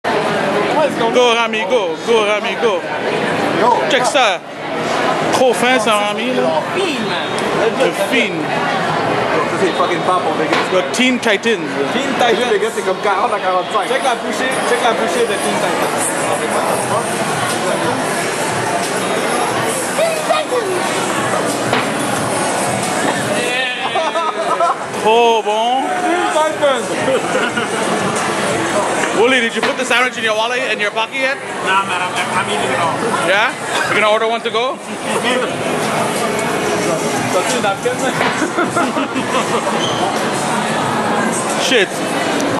Rami aka Teen Titan aka SugarFiend aka best UMVC3 player in canada
Rami destroying a cupcake from earl of sandwich, cameo by Woolie.

Evo 2012 Las Vegas